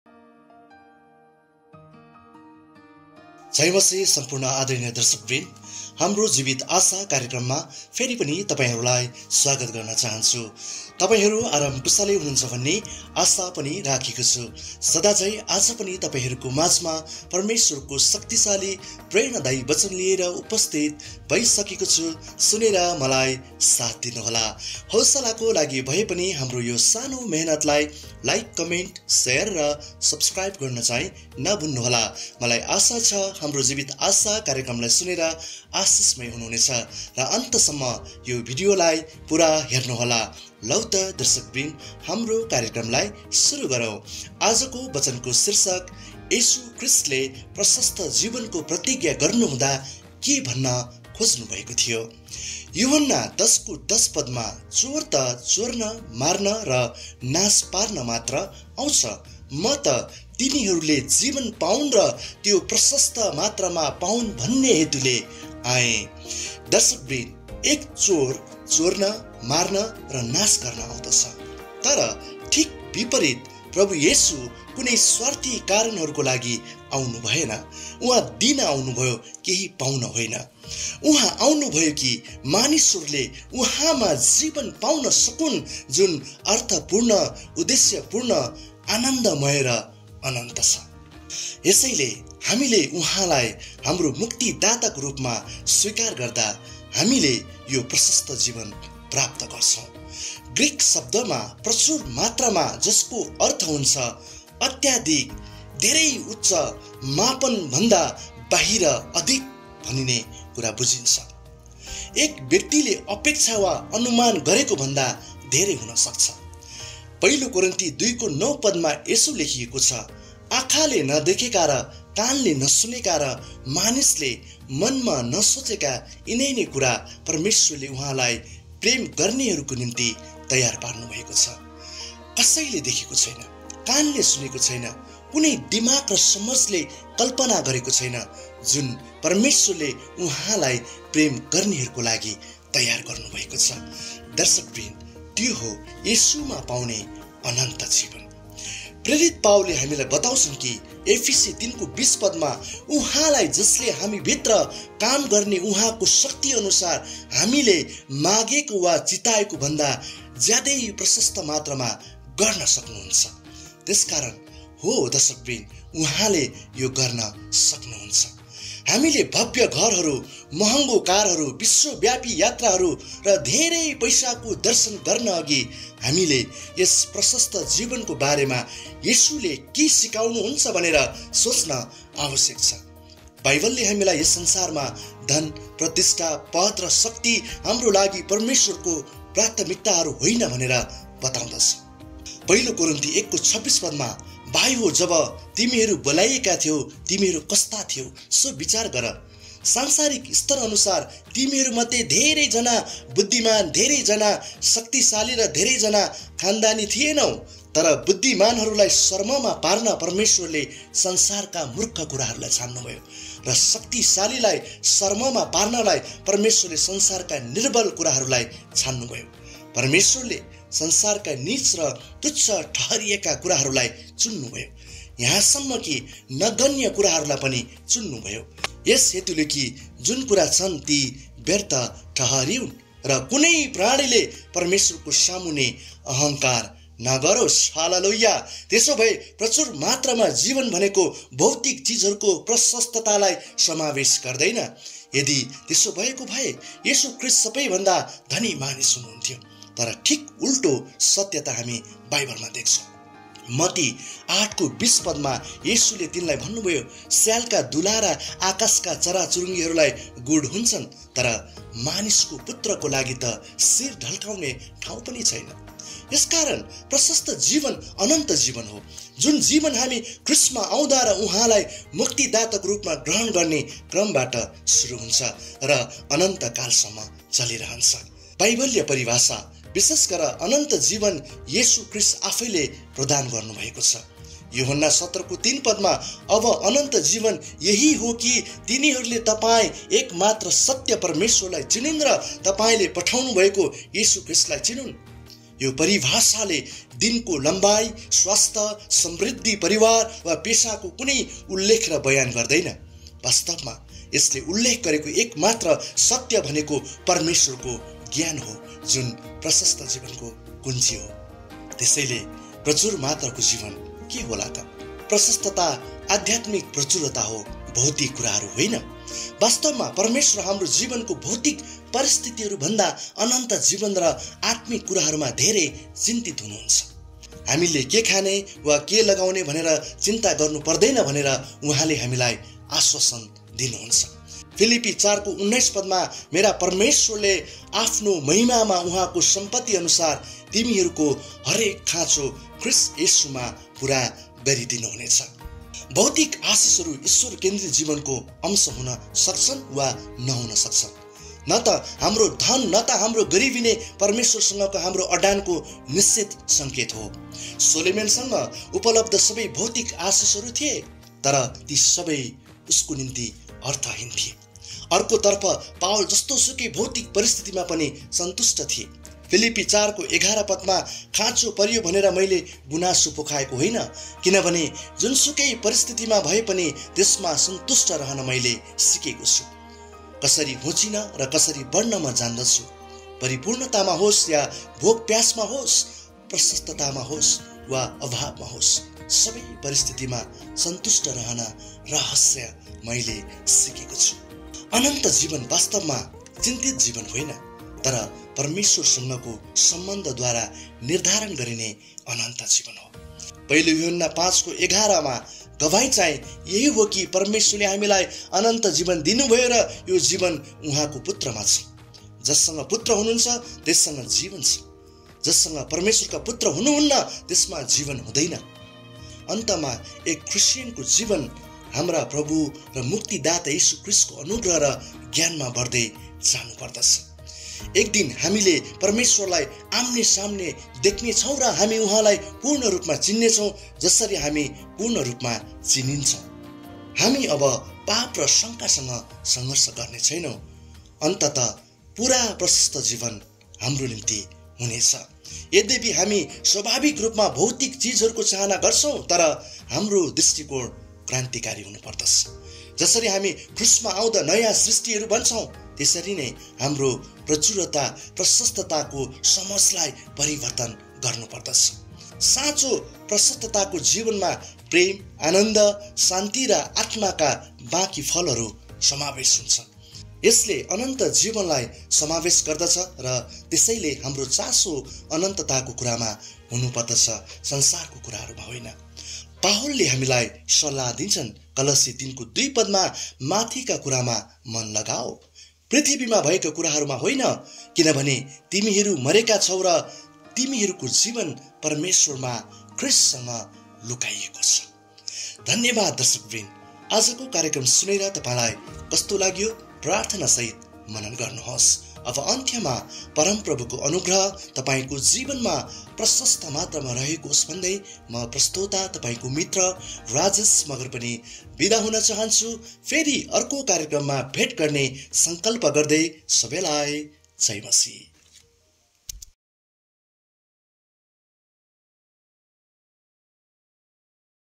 Eternal Life / अनन्त जीवन Program * Hamro Jibit Asha * online TV ministry Nepal.
येशूले प्रशस्त जीवनको प्रतिज्ञा गर्नुहुँदा के, भन्न खोज्नुभएको थियो?
यूहन्‍ना १०:१० “चोर त चोर्न, मार्न र नाश पार्न मात्र आउँछ। म त तिनीहरूले जीवन पाऊन्‌, र त्‍यो प्रशस्‍त मात्रामा पाऊन्‌ भन्‍ने हेतुले आएँ।
• दर्शकवृन्द, एक चोर चोर्न, मार्न र नाश गर्न आउदछ ।
• तर ठिक विपरीत, प्रभु येशू कुनै स्वार्थी कारणहरूको लागि आउनुभएन उहाँ दिन आउनुभयो केही पाउन होइन।
• उहाँ आउनुभयो कि मानिसहरूले उहाँमा जीवन पाउन सकुन जुन अर्थपूर्ण, उद्देश्यपूर्ण, आनन्दमय र अनन्त छ।
• यसैले हामीले उहाँलाई हाम्रो मुक्तिदाताको रूपमा स्वीकार गर्दा हामीले यो प्रशस्त जीवन प्राप्त गर्छौं।
• ग्रीक शश्दमा "प्रचुर मात्रामा"  जसको अर्थ "अत्यधिक, धेरै उच्च, मापन भन्दा बाहिर, अधिक भनने कुरा बुजिन्छ ।